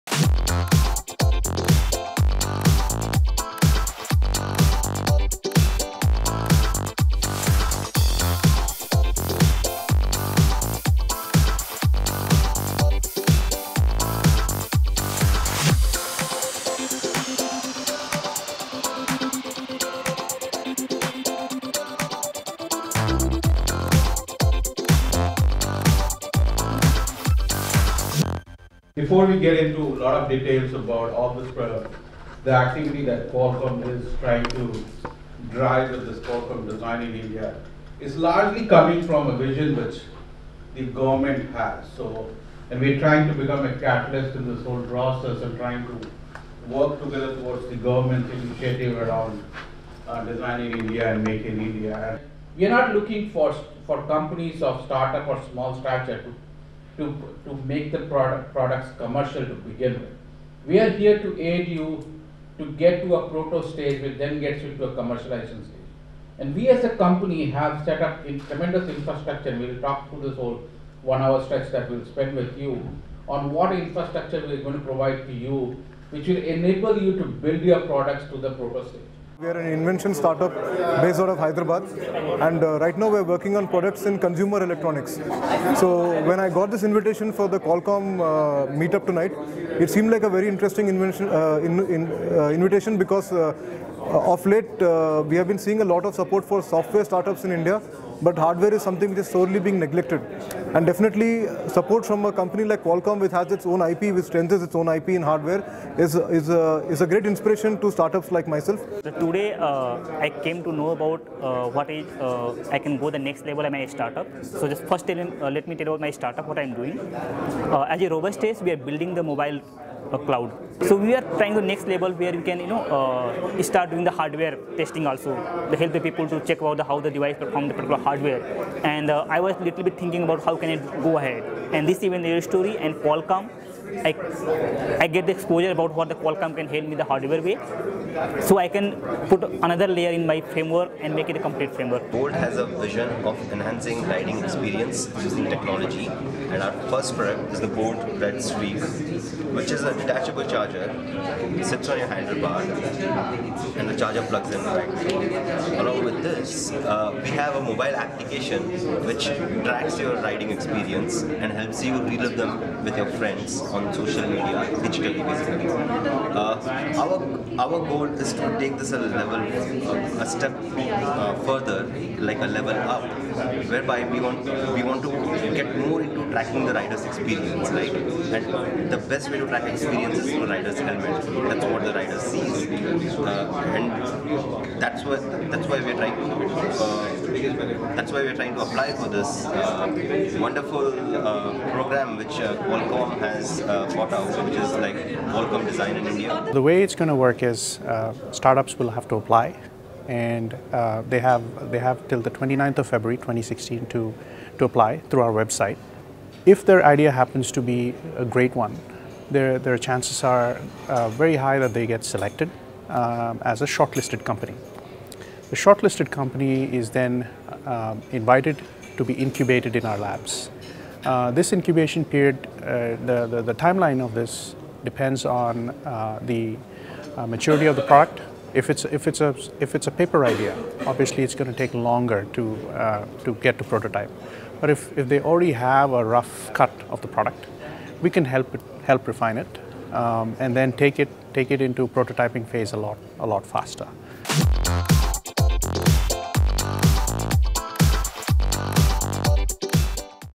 QUALCOMM - Support to the Hardware ecosystem
Being a hardware startup might have been difficult, but now being a hardware startup has become easy with the initiatives from Qualcomm